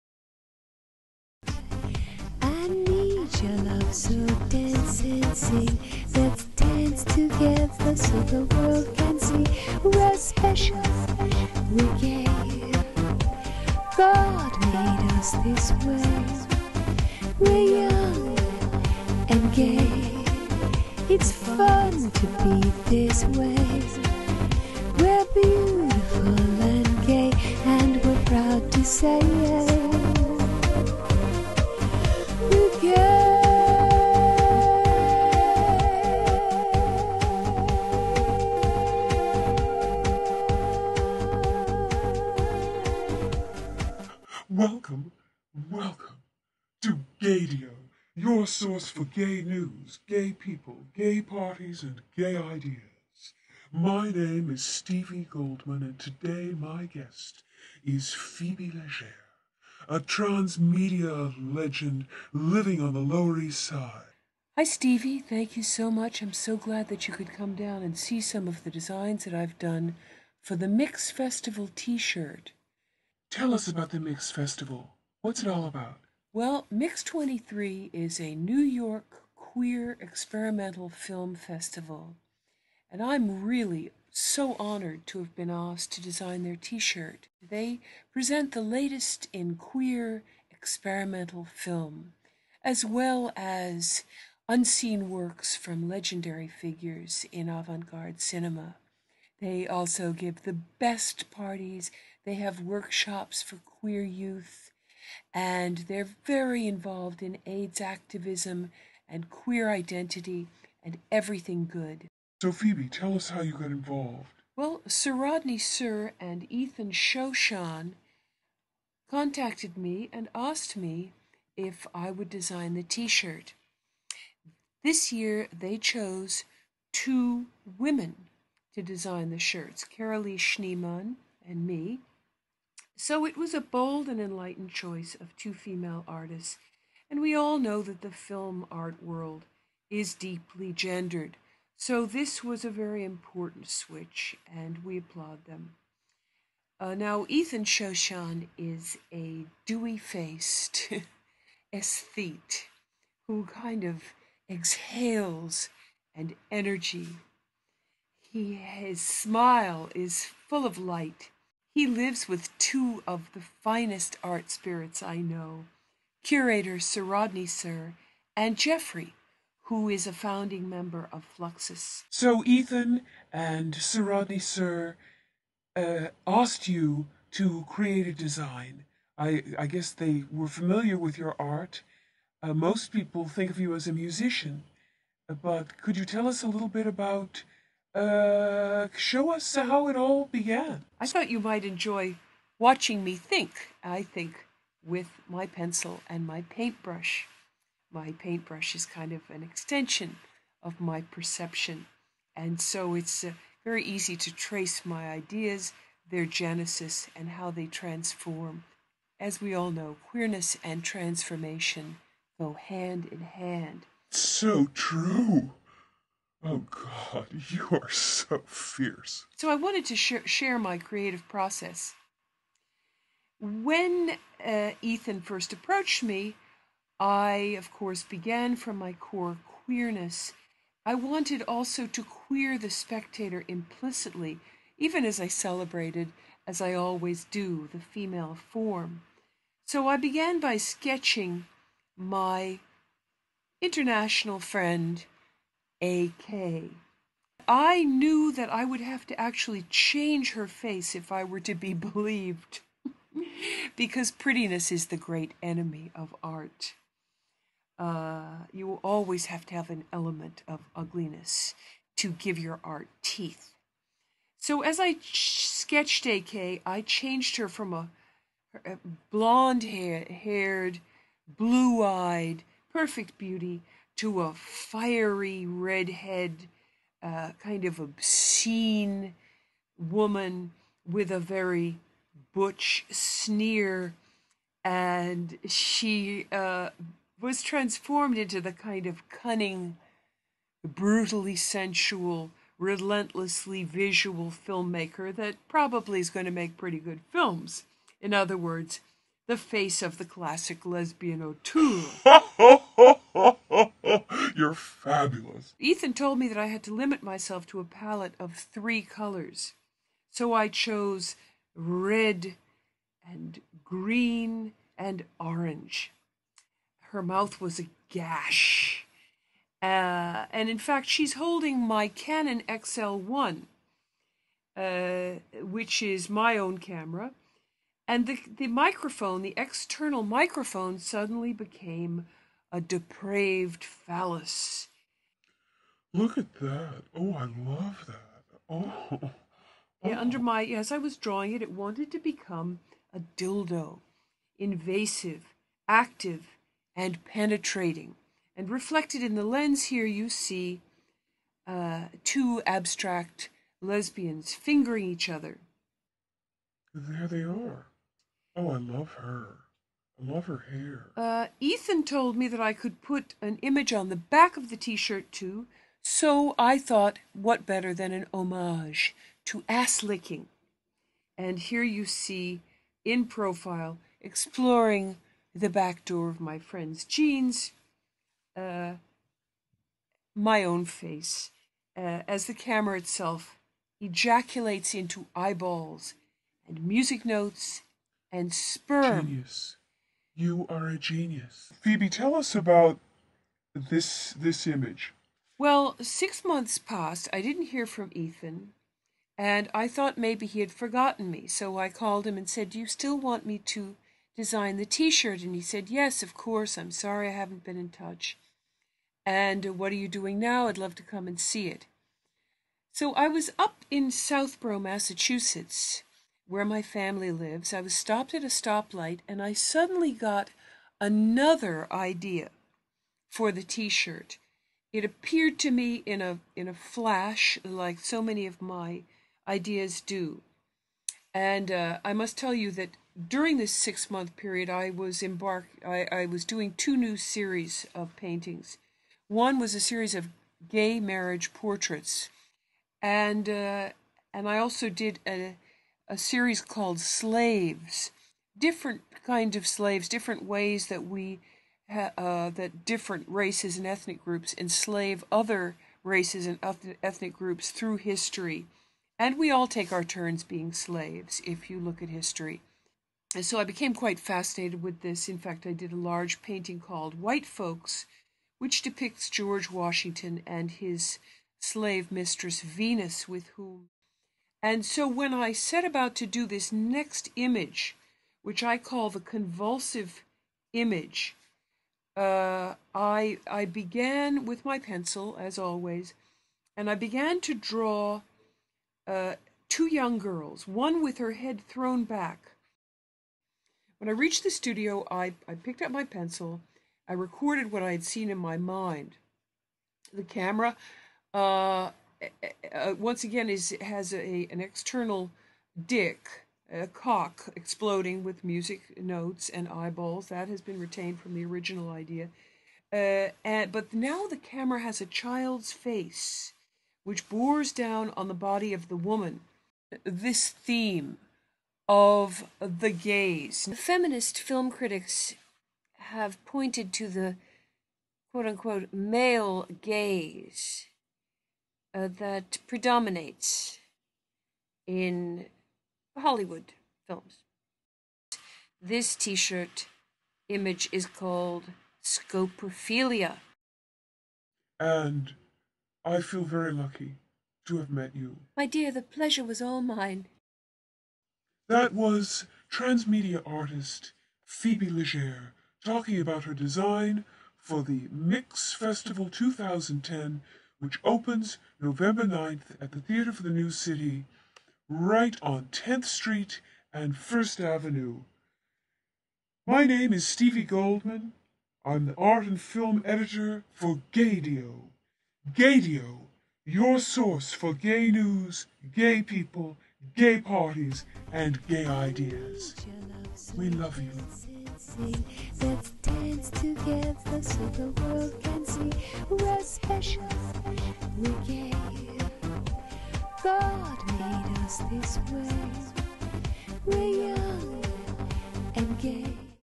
GAYDIO New York! Episode #1 Interview w/Phoebe Legere talking about THE MIX FESTIVAL 2010
Gaydio Art and Film Critic Stevie Goldman
interviews artist Phoebe Legere
about her designs for the
MIX 2010 Experimental Queer Film Festival T Shirt